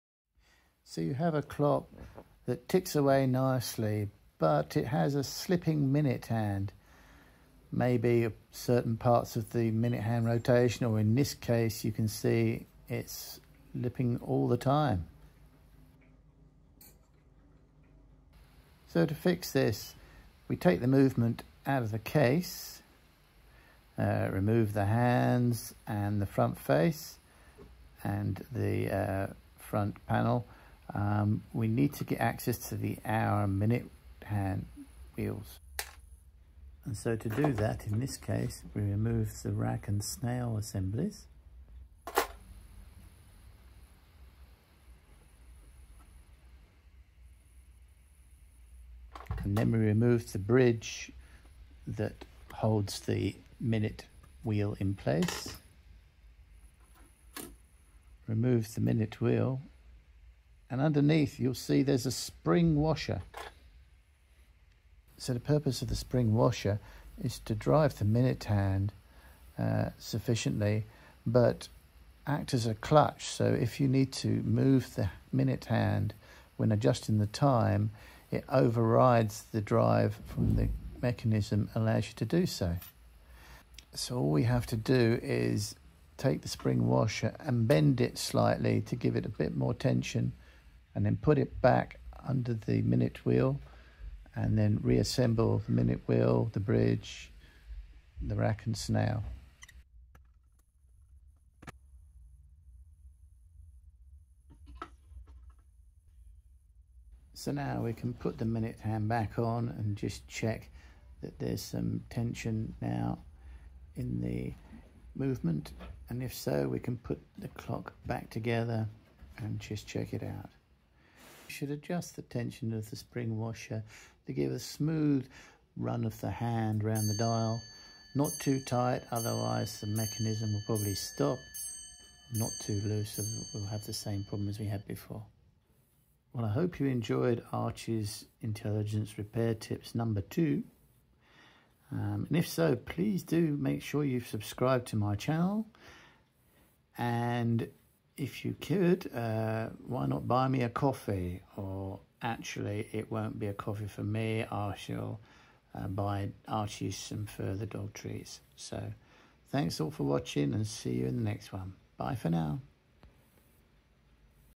Why Your Pendulum Clock's Minute Hand Keeps Slipping
Sometimes a clocks minute hand does not rotate smoothly or can become totally loose. The clock still ticks however and so we will have to investigate further!
The clock hands depend on the tension washer to rotate smoothly when driven by the clock mechanism, and the washer acts like a clutch so when you adjust the clocks time by moving the minute hand, it overrides the clock mechanism without damaging the movement.
Sometimes the tension washer loses its spring and the hands become loose, so this video shows you how to correct for this. Enjoy!